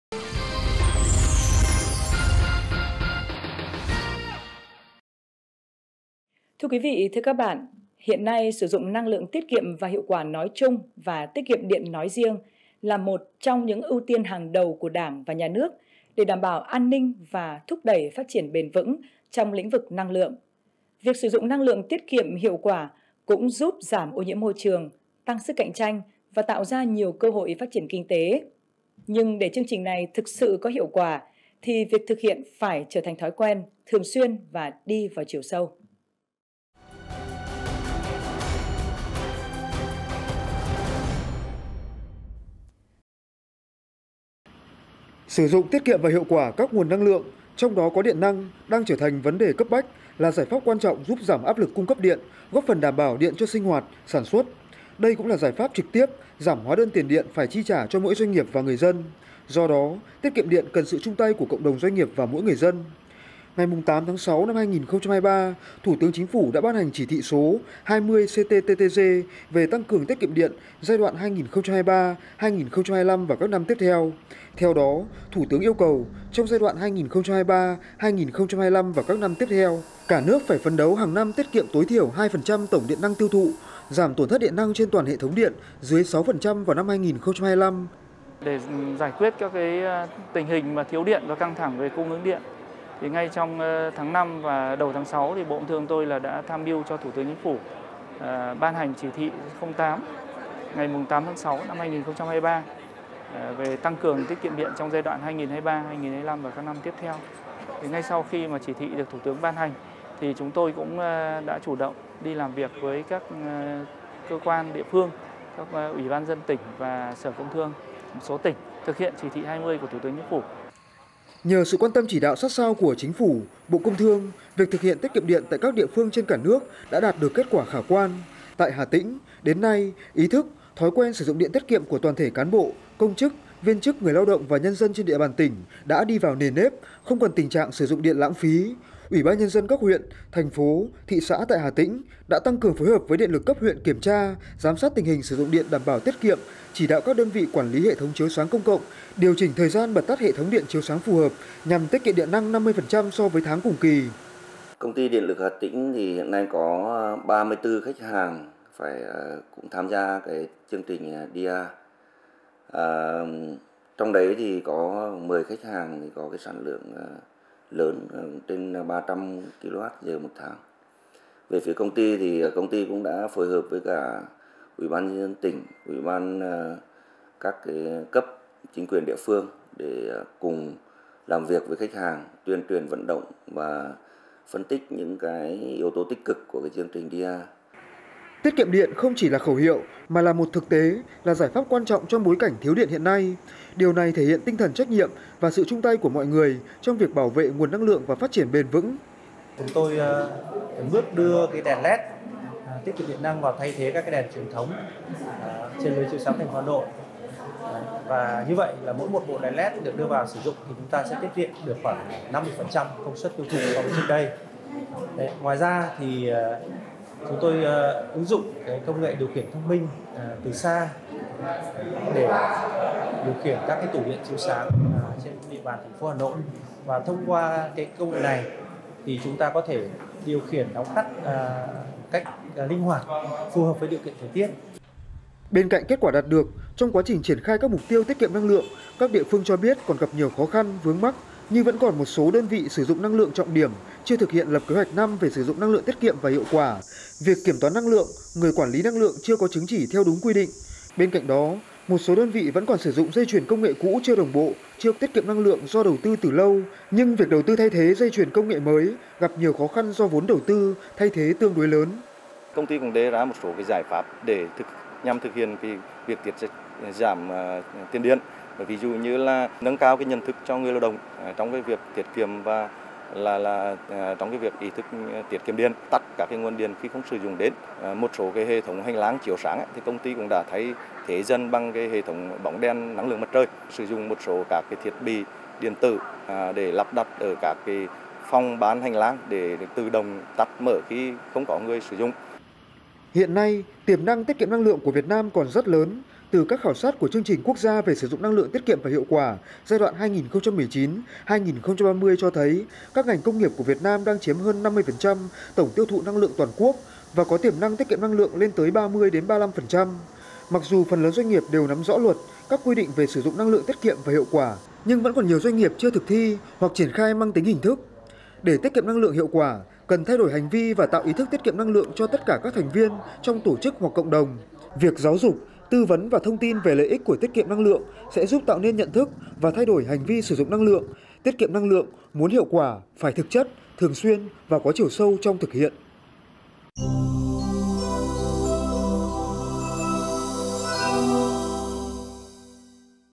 Tiết kiệm điện phải thành thói quen, thực hiện thường xuyên, hiệu quả
Hiện nay, sử dụng năng lượng tiết kiệm và hiệu quả nói chung và tiết kiệm điện nói riêng, là một trong những ưu tiên hàng đầu của Đảng và Nhà nước để đảm bảo an ninh và thúc đẩy phát triển bền vững trong lĩnh vực năng lượng. Việc sử dụng năng lượng tiết kiệm, hiệu quả cũng giúp giảm ô nhiễm môi trường, tăng sức cạnh tranh và tạo ra nhiều cơ hội phát triển kinh tế. Nhưng để chương trình này thực sự có hiệu quả, thì việc thực hiện phải trở thành thói quen, thường xuyên và đi vào chiều sâu...